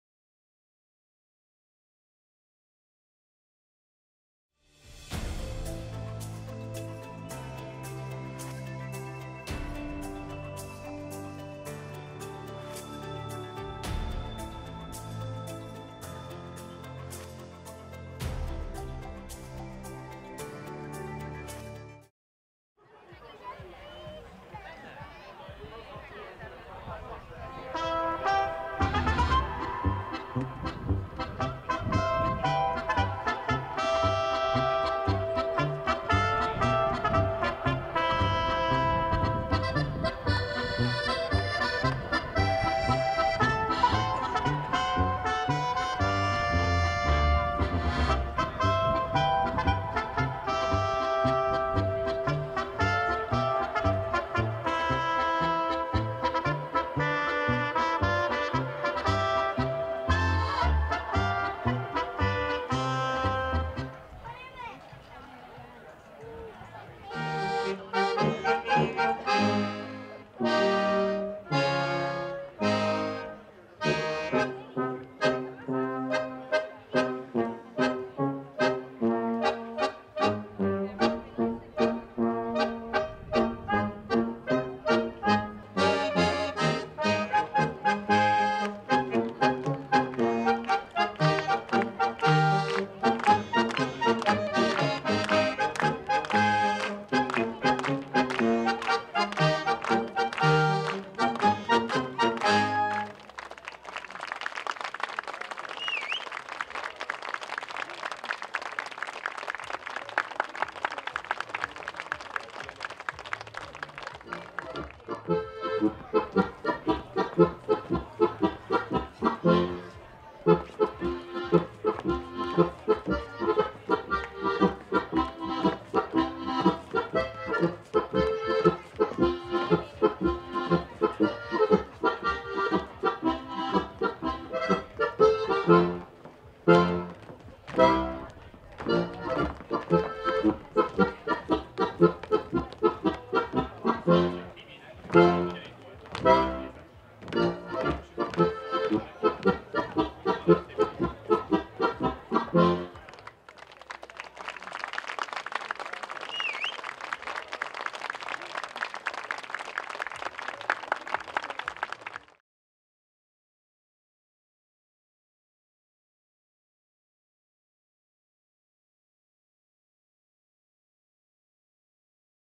Biatorbágyi Majális 2023-A Füzes Néptáncegyüttes gyerekcsoportjának fellépése(23.05.04.)-Völgyhíd TV
Idén is jó hangulatban, számos különféle programon vehettek részt a biatorbágyiak városunk idei első igazi tavaszi tömegrendezvényén, a Füzes-patak partján megrendezett városi Majálison.

Az egész napos rendezvényen a Füzes Néptáncegyüttes gyerekcsoportja is fellépett.